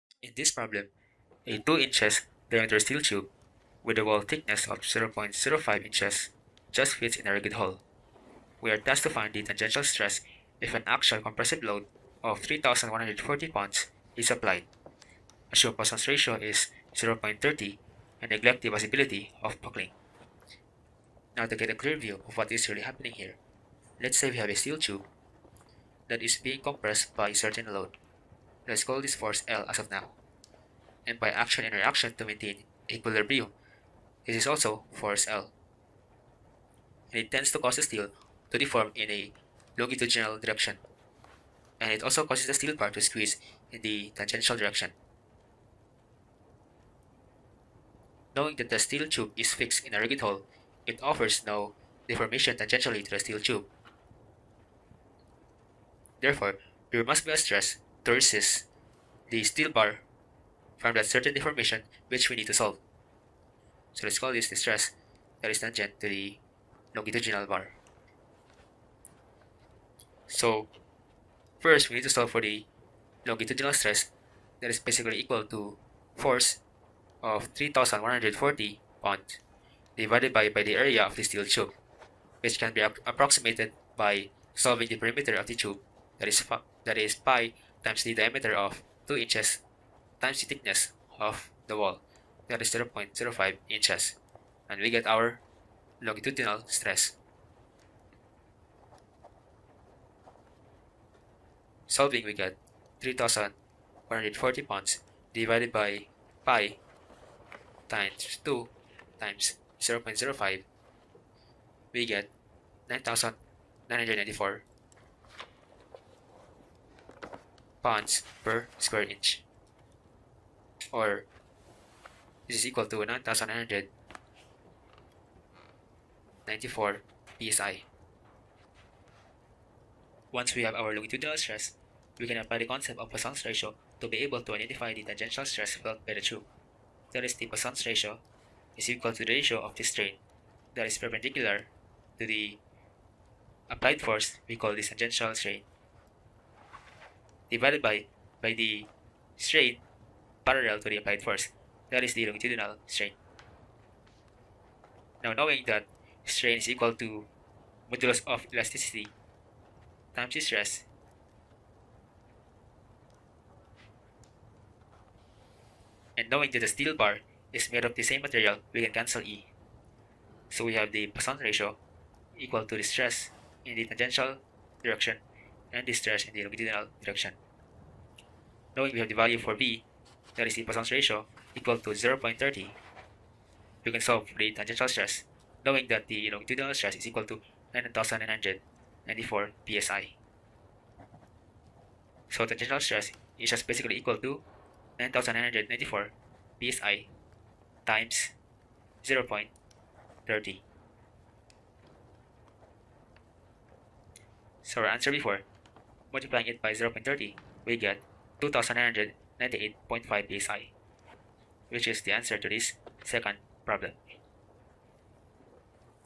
Strength of Materials - Sample Problem 2 (Biaxial Deformation of a Steel Tube)
2. A 2-in.-diameter steel tube with a wall thickness of 0.05 inch just fits in a rigid hole. Find the tangential stress if an axial compressive load of 3140 lb is applied. Assume Poisson's ratio is 0.30 and neglect the possibility of buckling.